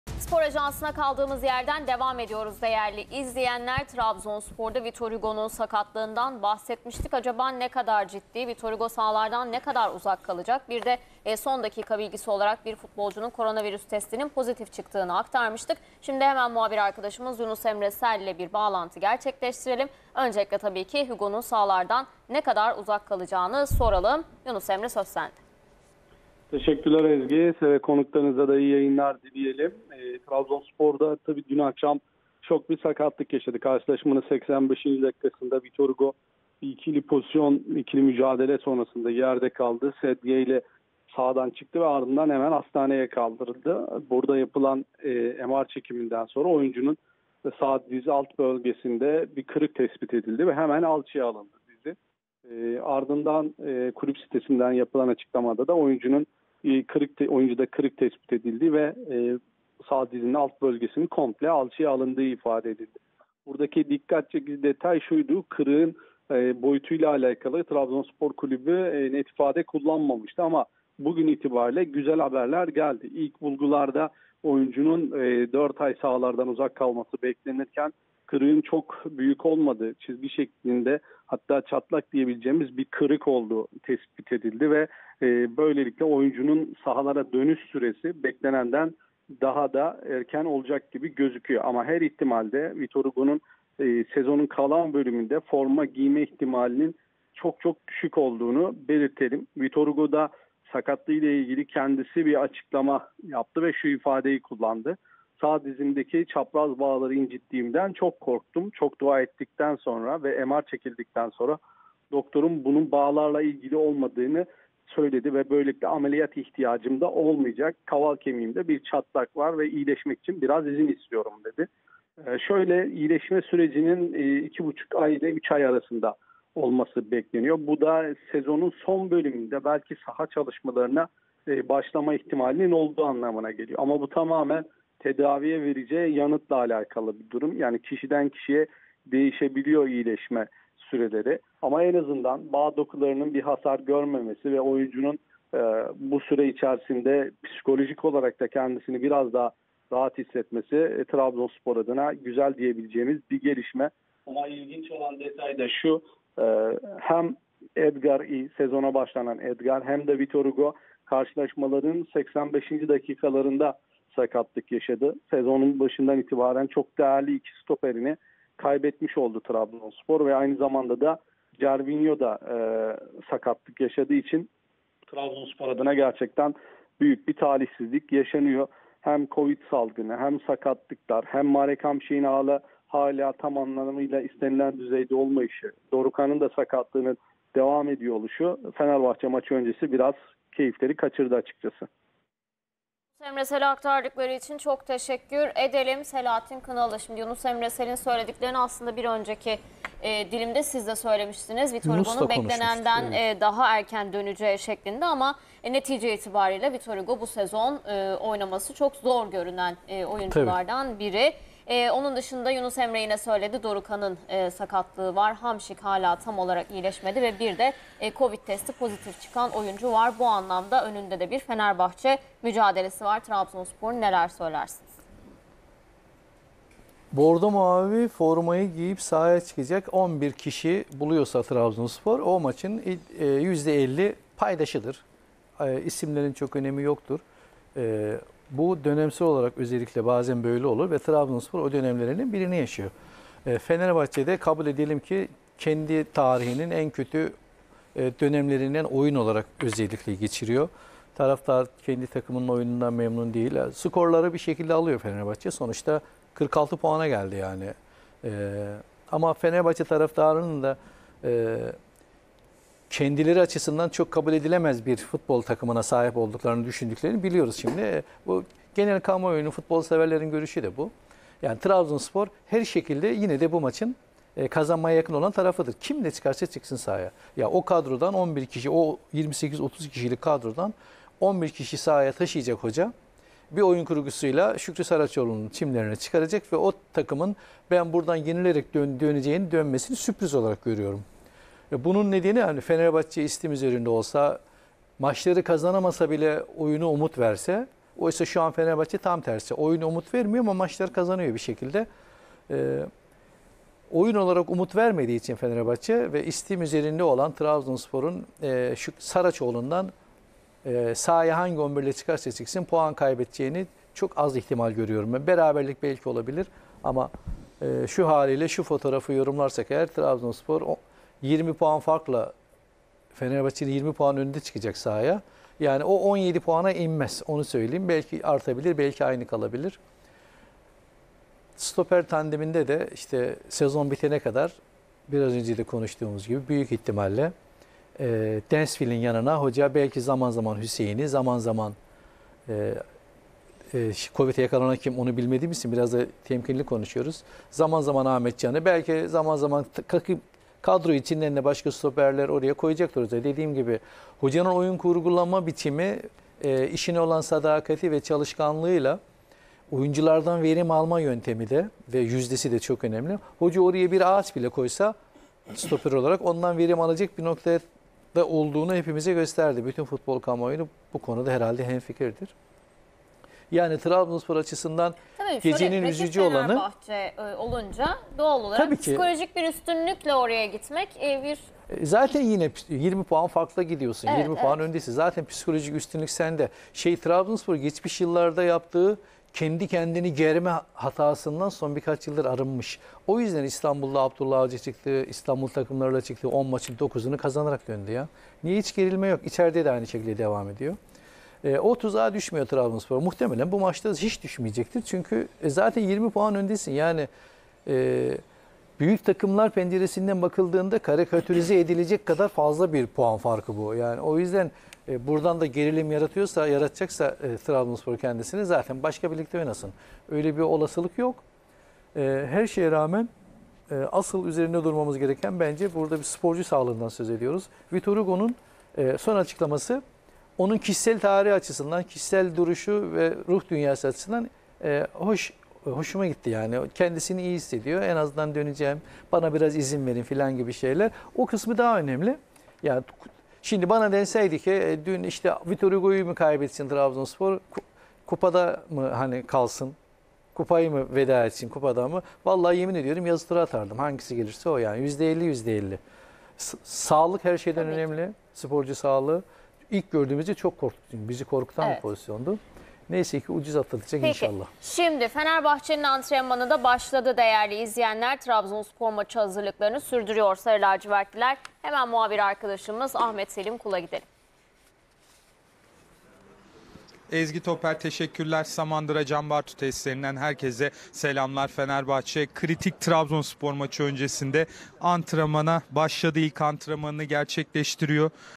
Vitor Hugo Sahalaradan Ne Kadar Uzak Kalacak? Yunus Emre Sel Açıkladı / A Spor / Spor Ajansı
Vitor Hugo Sahalaradan Ne Kadar Uzak Kalacak? Yunus Emre Sel Açıkladı / A Spor / Spor Ajansı / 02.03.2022